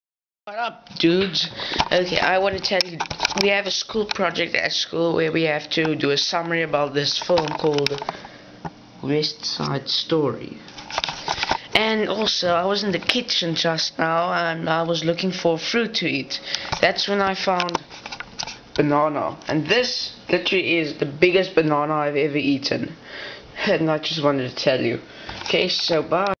Simon and the Giant Banana
I was in the kitchen looking for a fruit to eat,when I saw it. The biggest banana ever! Keep this away from Gru's minions! lol XD